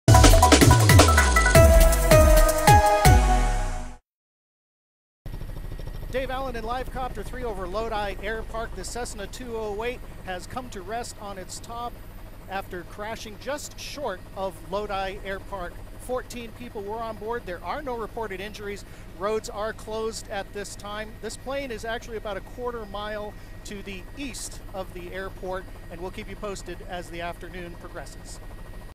VIDEO: Skydiving plane crashes near Lodi airport
A skydiving plane landed upside down Thursday afternoon in a field near the Lodi airport.